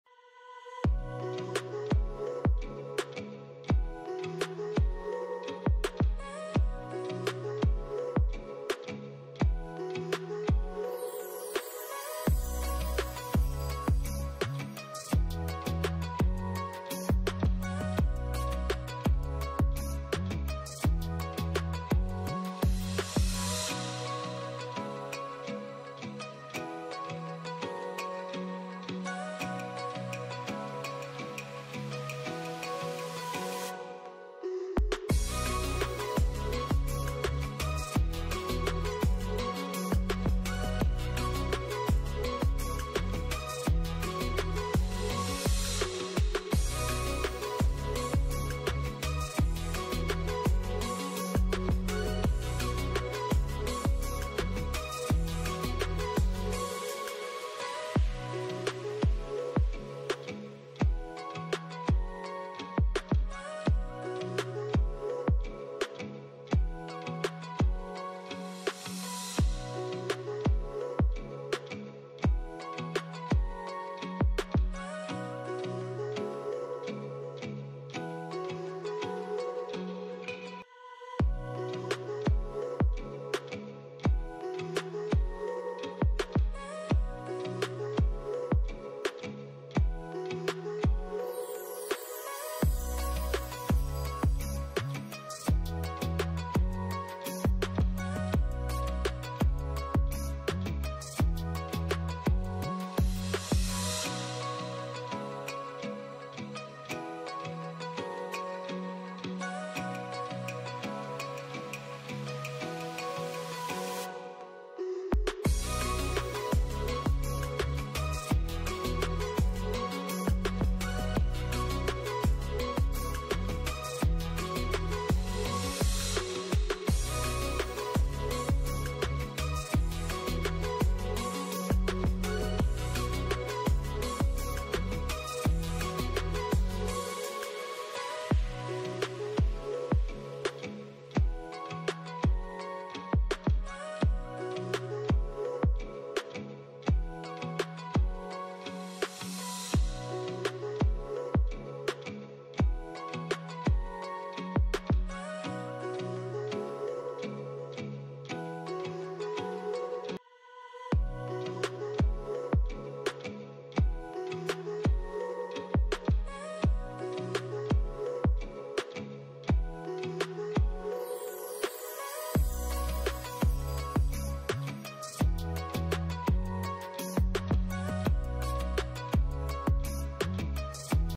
como hacer un moño sencillo de liston
Hola en este mini tutorial te mostrare como hacer fácilmente un moño sencillo de liston